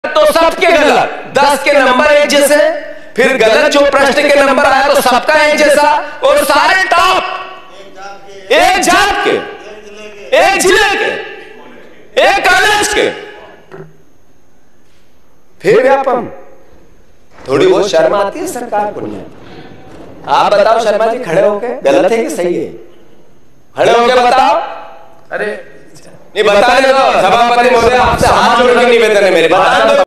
Jitu Patwari Speech MP VYAPAM घोटाला । मध्य प्रदेश व्यापम घोटाला परीक्षा प्रणाली के ऊपर ।
जीतू पटवारी जी ने मध्य प्रदेश परीक्षा प्रणाली के ऊपर अपने विचार व्यक्त किए ।

मध्य प्रदेश व्यापम घोटाला ।